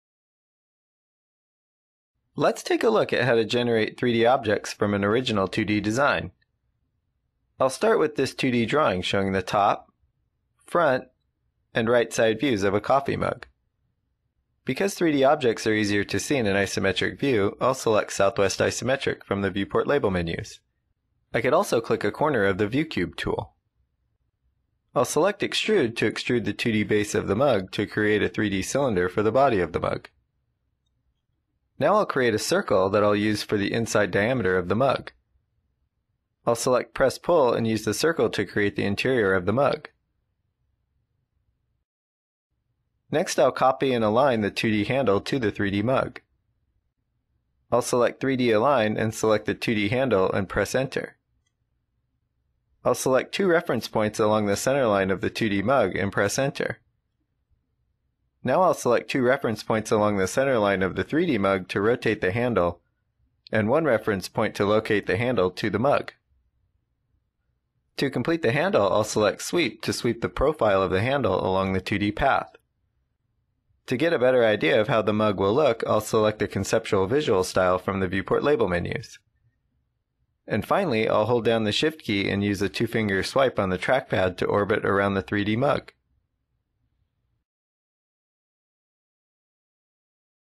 Convert 2D Objects to 3D Objects: AutoCAD 2013 for Mac
Many 3D models start out as 2D profiles that are extruded, lofted, revolved, or swept into 3D space. This video provides an overview of how to create 3D objects from 2D objects.